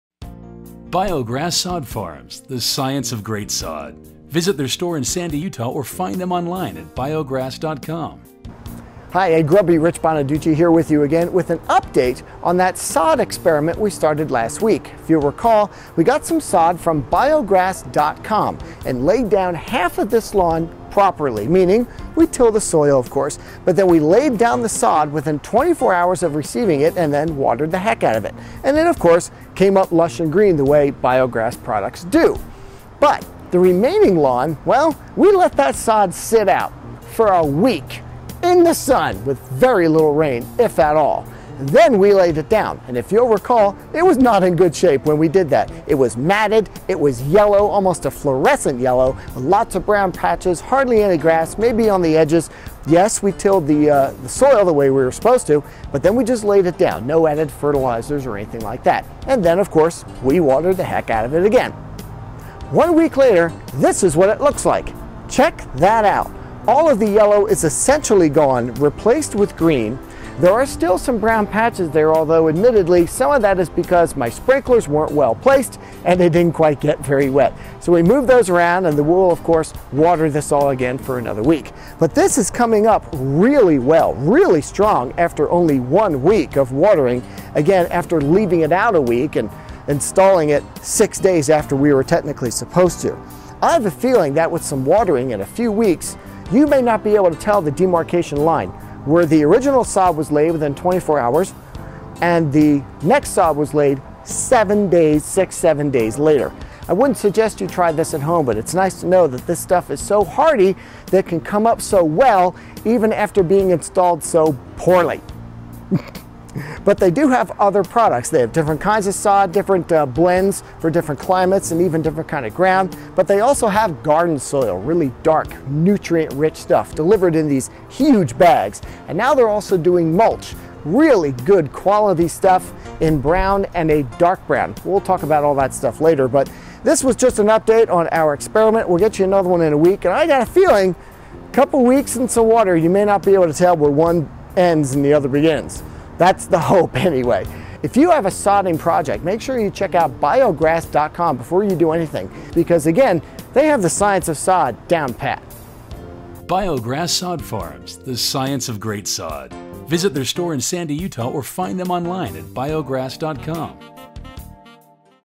Double Secret BIOGRASS Experiment, Week 2
Two weeks ago, we let half of our BIOGRASS sod sit out on pallet, baking in the sun for a WEEK. Then we laid it down next to BIOGRASS sod that has been in place for years. Let's see what happens!